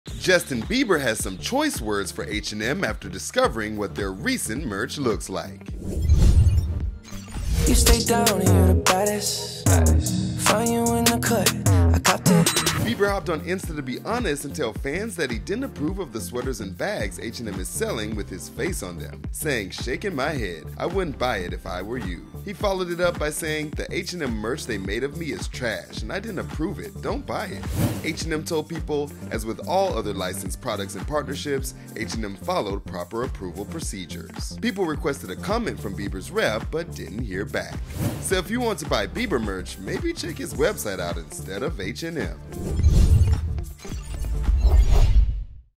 Justin Bieber Calls Out H&M For Releasing Merch Collection Without His Permission | Billboard News
The singer took to Instagram to react to the H&M merch release.